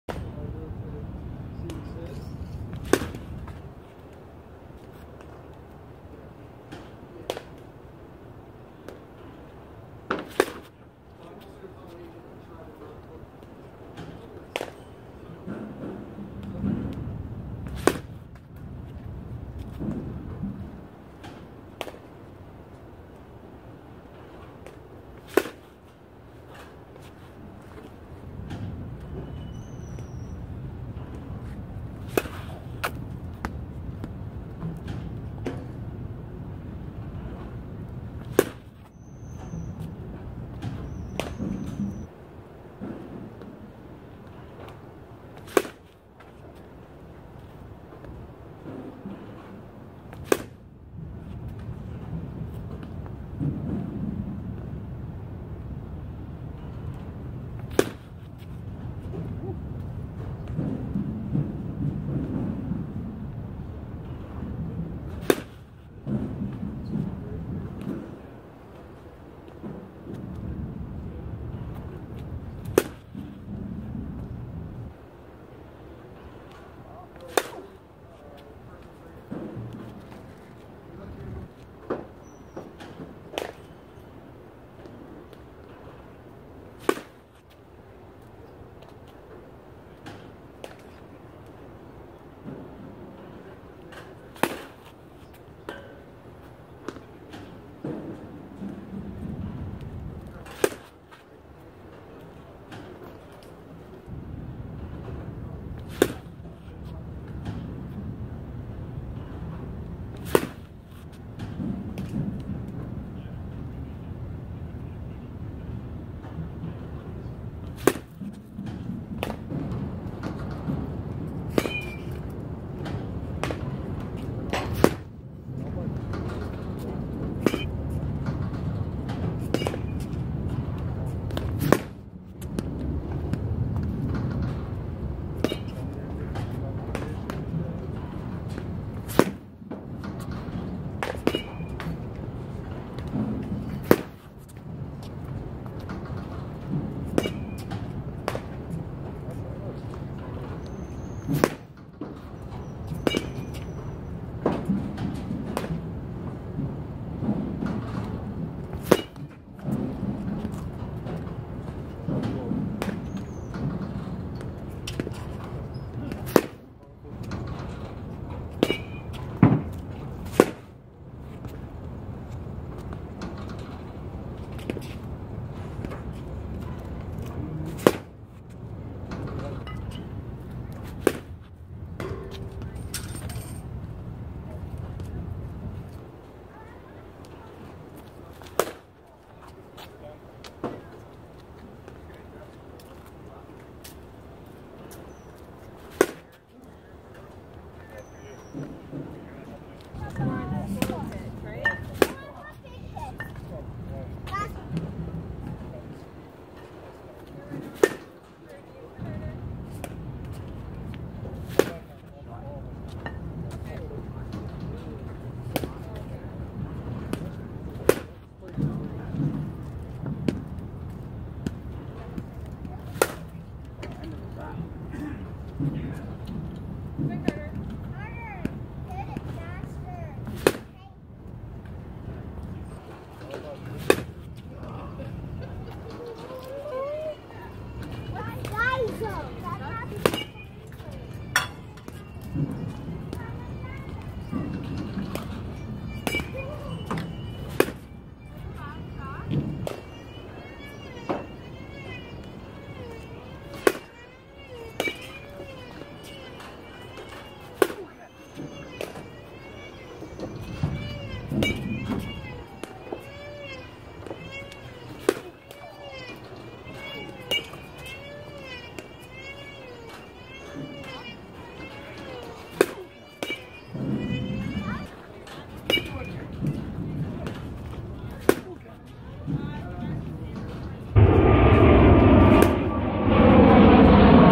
Louisville Slugger XXL Cuts
Doing Cuts with the Louisville Slugger XXL. Note I am using this bat as an exclusive Mush Ball Bat as I have Dome Ball up North in the winter.  This was taken at Diamond Beach where they use Mush Balls at their Cages